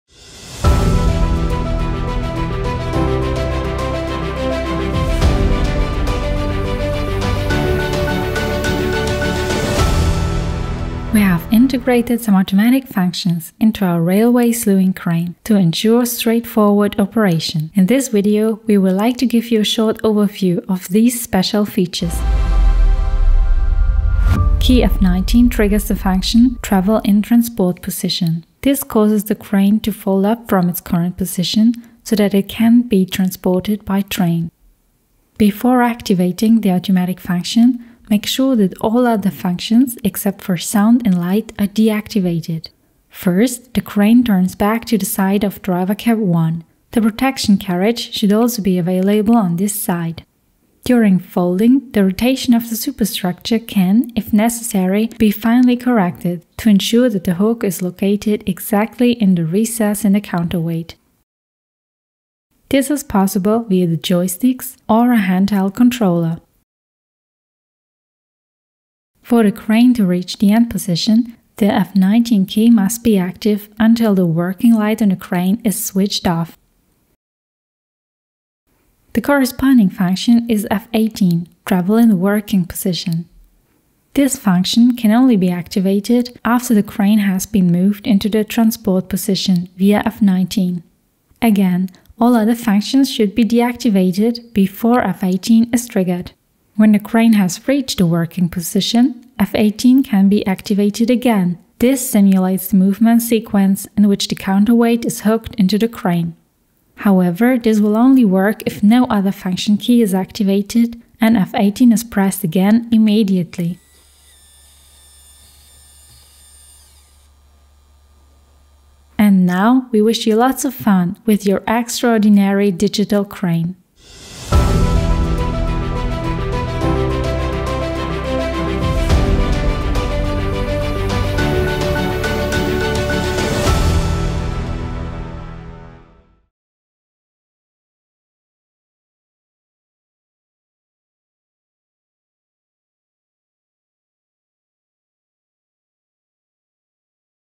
EDK 750 Automatic functions - Tutorial Part 1
You have several possibilities to control our railway slewing crane EDK750.
In this video we will show you how to best operate it, either by using a handheld controller, the Z21 app or a PS4™-Controller.